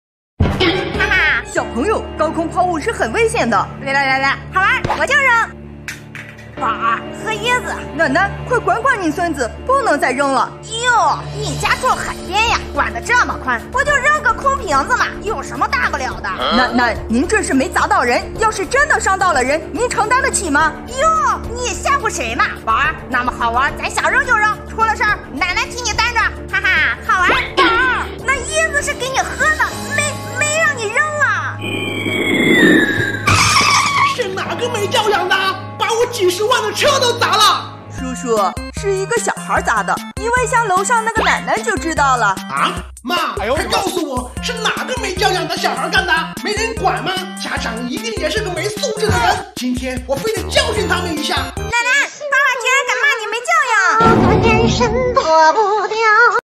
【动漫】奶奶纵容孙子行为太过分，还好屁登机灵，结局自食其果
►酥饼卡通发布的视频，本页面汇集、整理酥饼卡通在上发布的高清在线视频